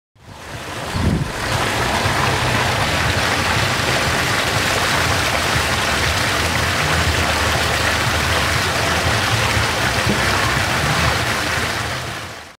Fountain Hurghada Seagull Beach Resort
A beautiful early morning view of our fountain in the grounds of our hotel...